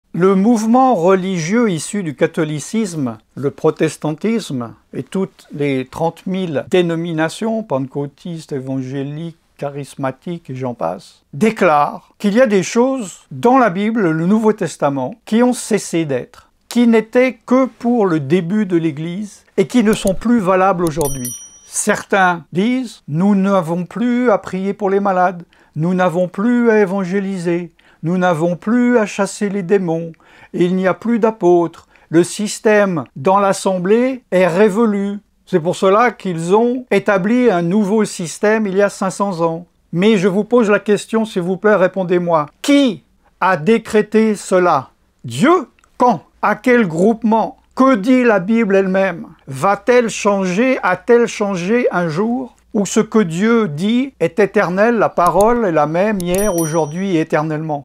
QUI A RÉVOQUÉ CES VERSETS? DIEU OU L’HOMME? Allan Rich
QUI A INVALIDÉ CES VERSETS DU NOUVEAU TESTAMENT? DIEU OU L’HOMME? Allan Rich
Quand et surtout qui a pris l’autorité d’abroger, d’ôter et de mettre des éléments du nouveau testament? Ceux qui prétendent et proclament être les seuls à y obéir! Allan Rich